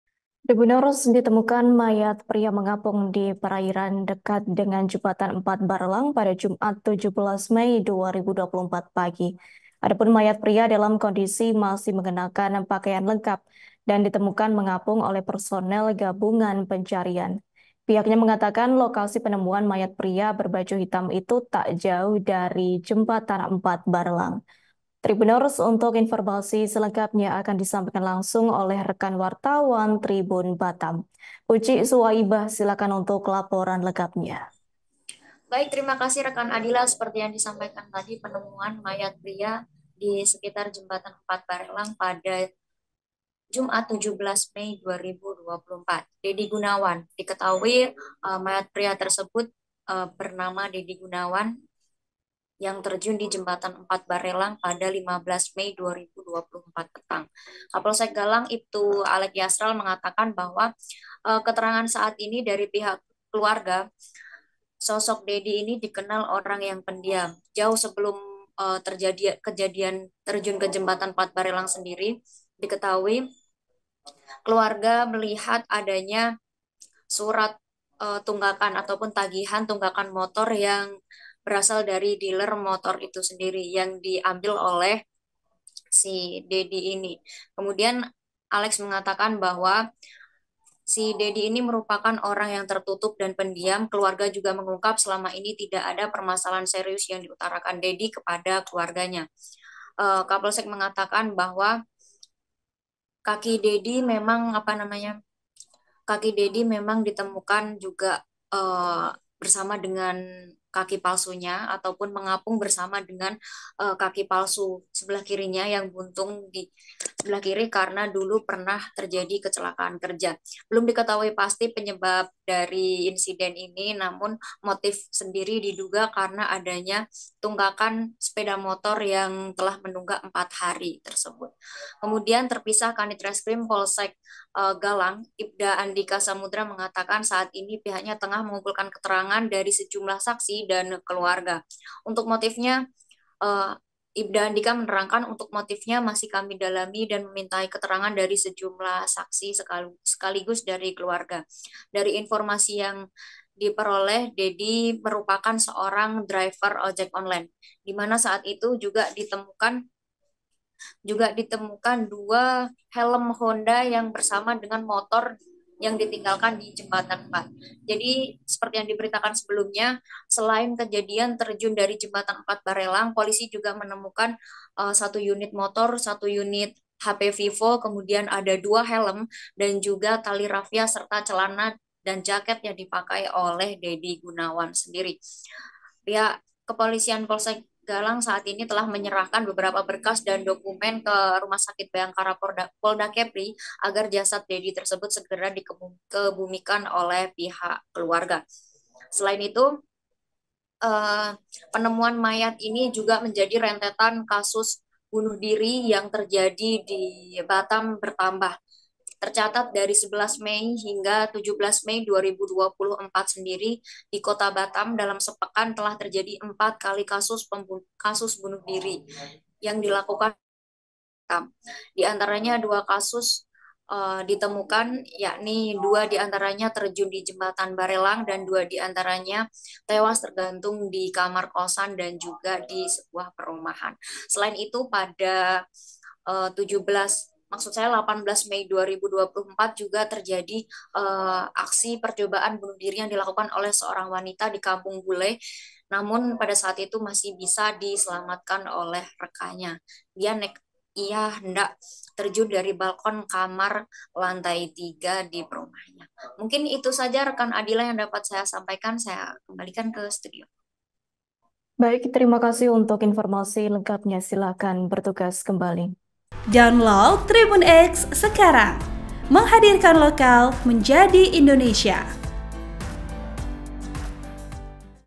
Mayat Pria Ditemukan Mengapung di Perairan Dekat Jembatan 4 Barelang Batam, Kondisi Masih Utuh
Laporan wartawan Tribun Batam-Ucik Suwaibah
TRIBUN-VIDEO.COM- Penemuan mayat pria mengapung di perairan dekat dengan jembatan 4 Barelang pada Jumat, (17/5/2024) pagi.

Mayat pria dalam kondisi masih mengenakan pakaian lengkap, tersebut ditemukan mengapung oleh personel gabungan pencarian.

Ia mengatakan lokasi penemuan mayat pria berbaju hitam ini tak jauh dari jembatan 4 Barelang. (*)